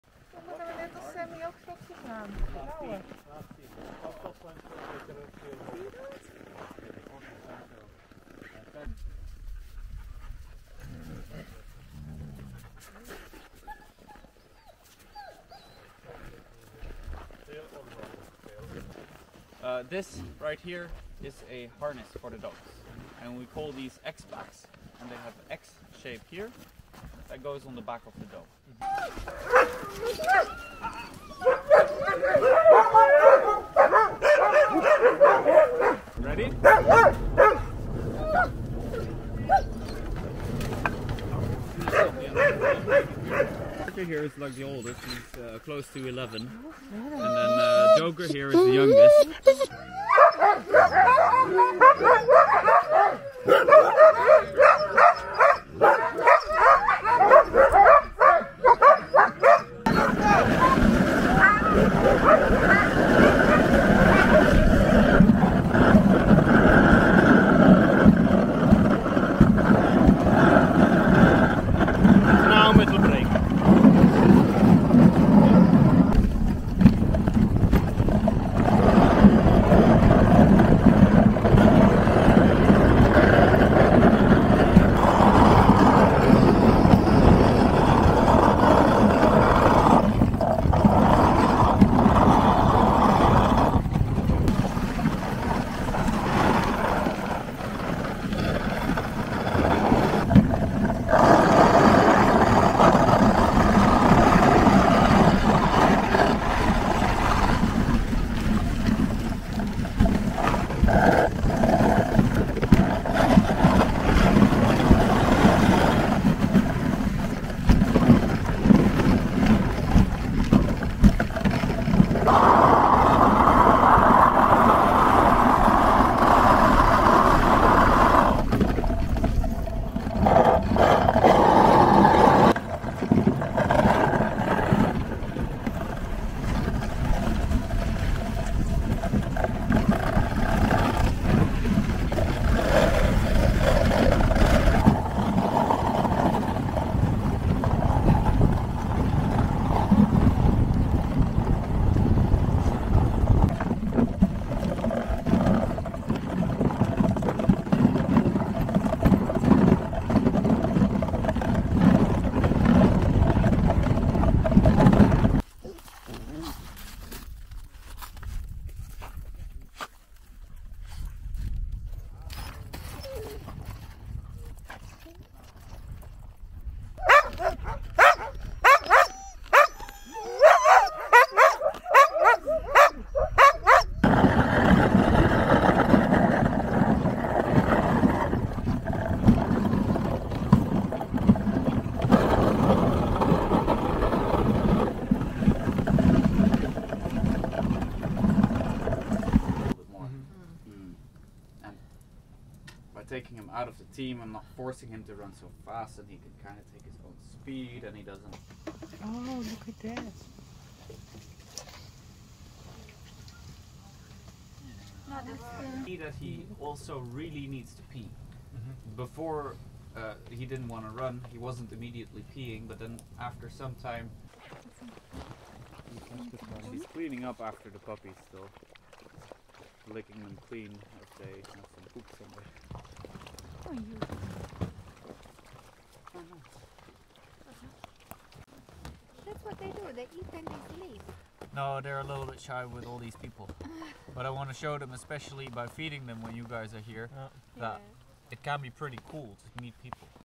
Kiruna Husky - Be a musher daytour
Impression of the Be a Musher daytour, organised by Kiruna Husky. A great adventure that ticks off one of the boxes on your bucket list: do a husky sled tour in Lapland!